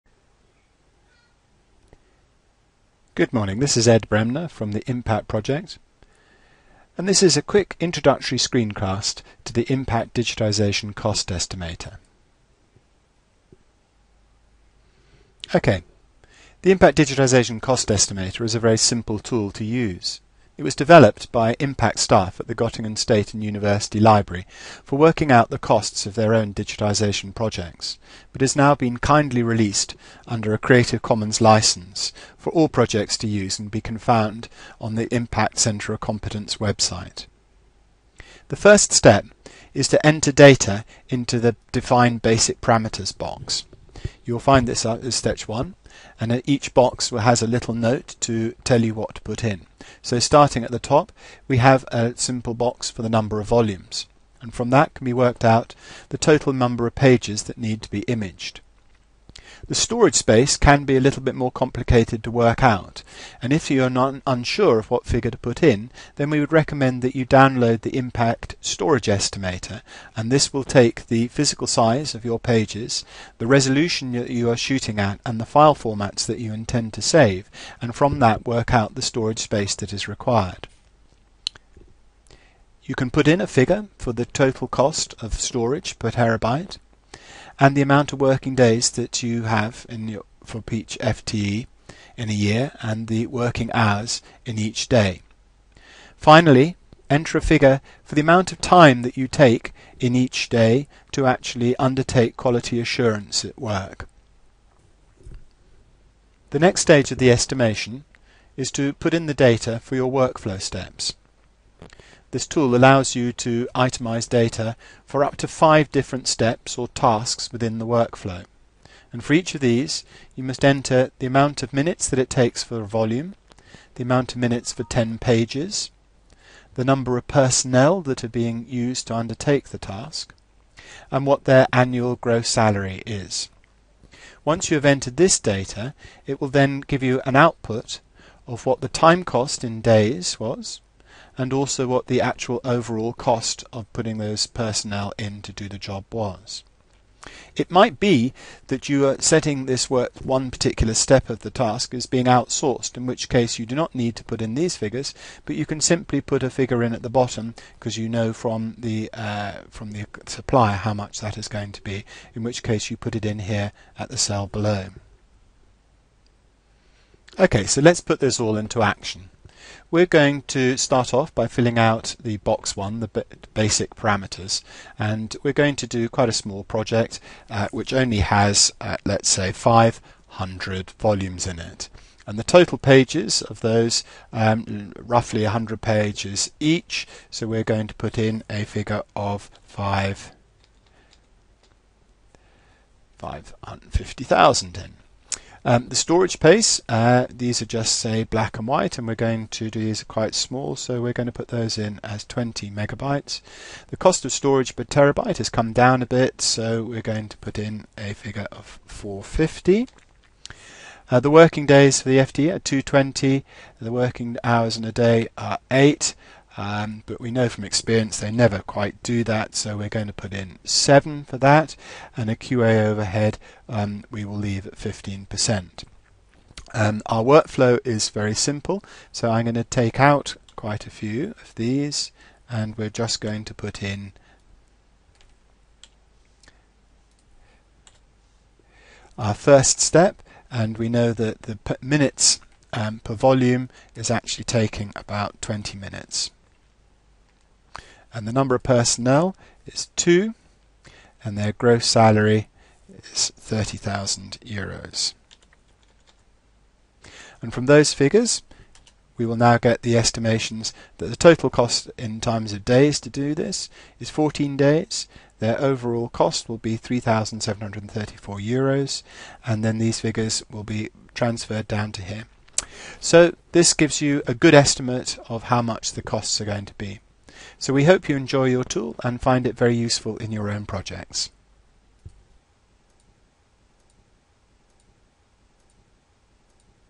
IMPACT Digitisation Cost Estimator - Screencast
Short Introduction screencast to the IMPACT Digitisation Estimator